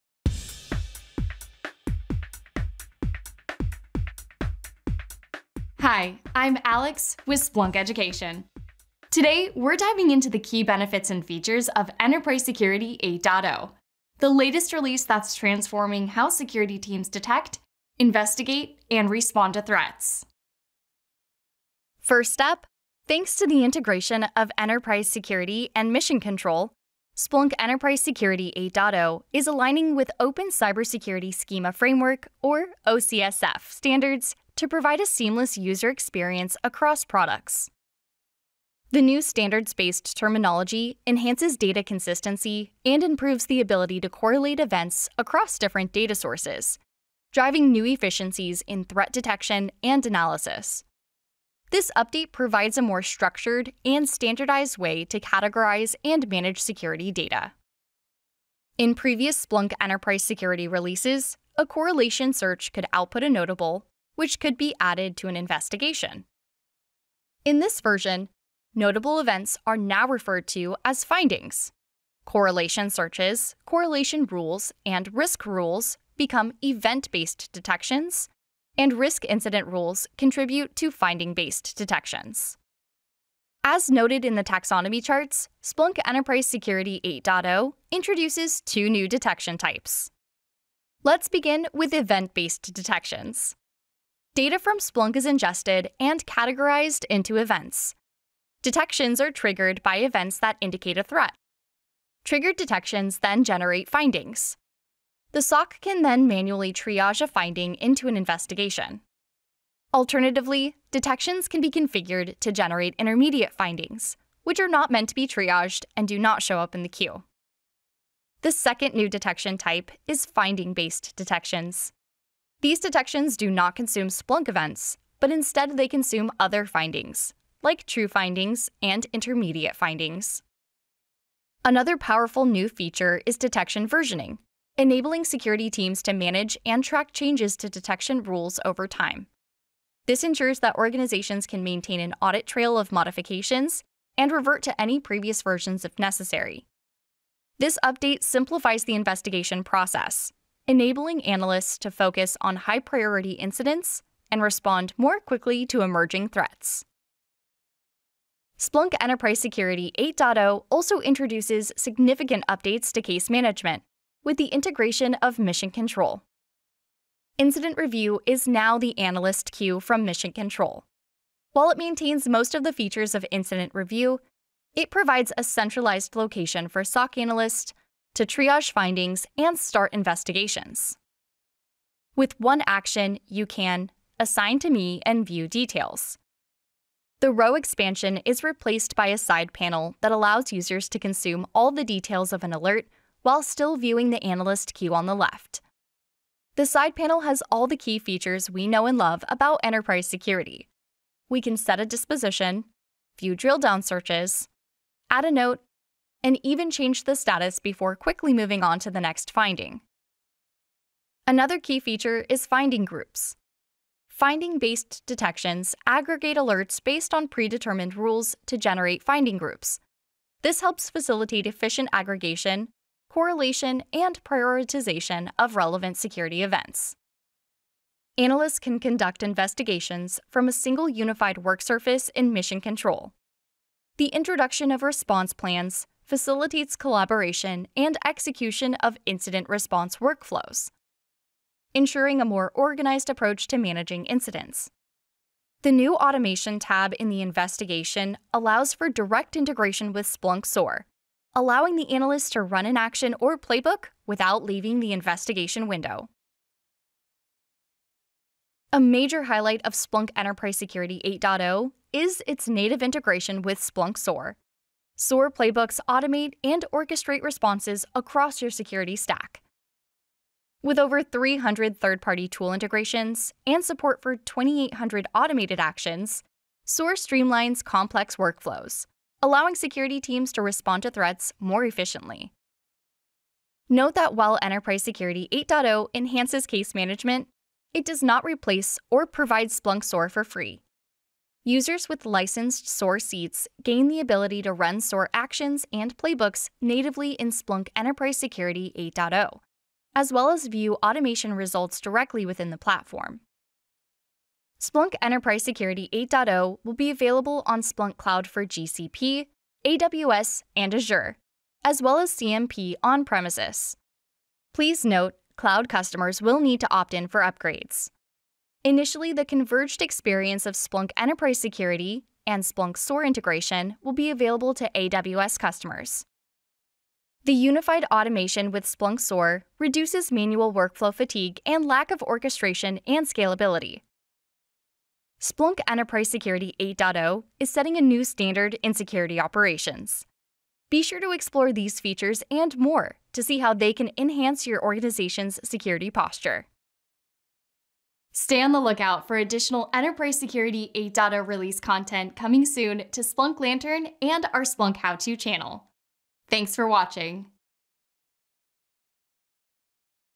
Splunk Enterprise Security 8.0 Workflows
Discover the key benefits and features of Splunk Enterprise Security 8.0, the latest release that's transforming how security teams detect, investigate, and respond to threats.